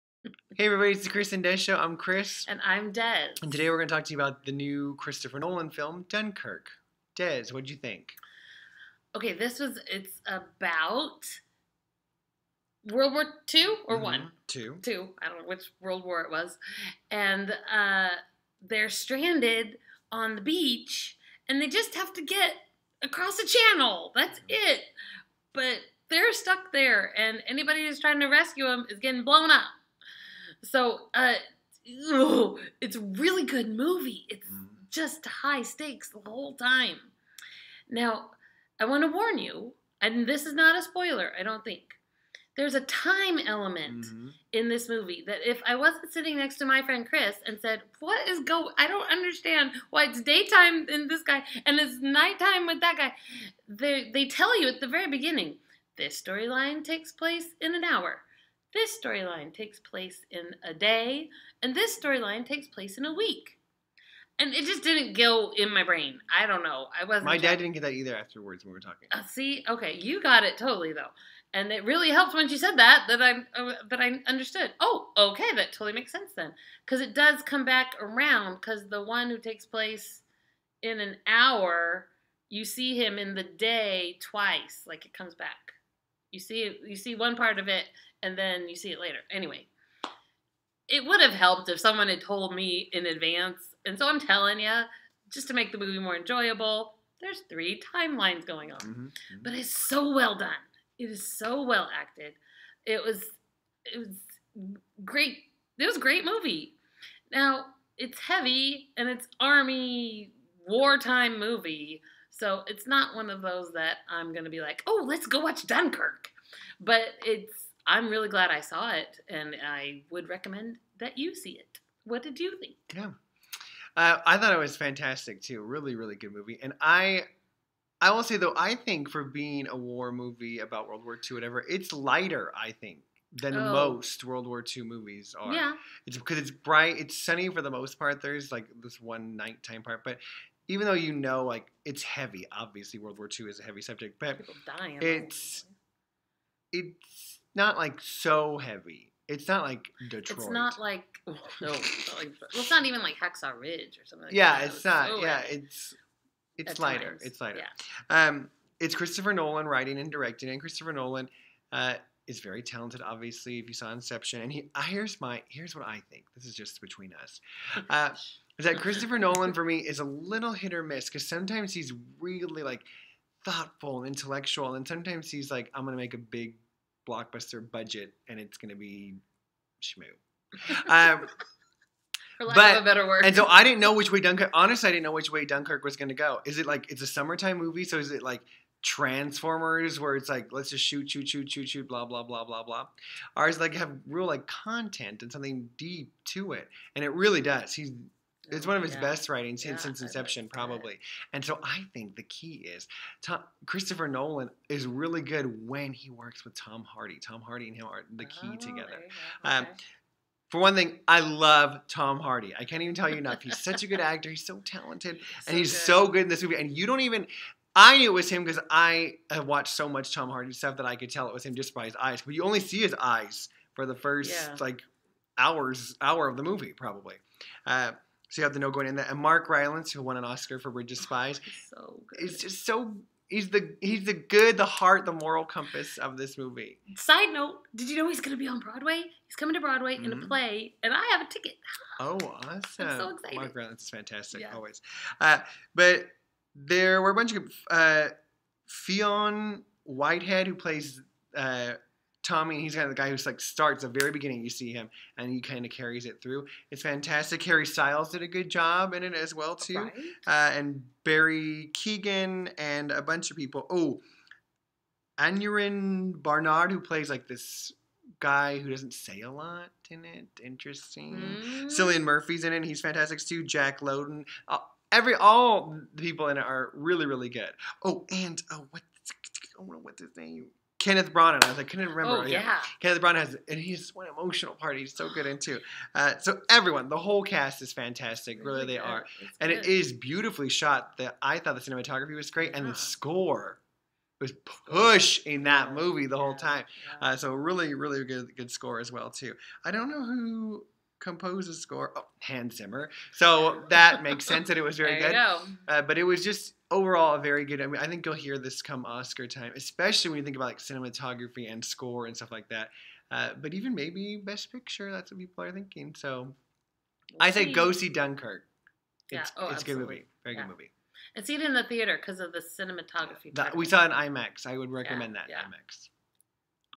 Dunkirk
We review the film Dunkirk written and directed by Christopher Nolan!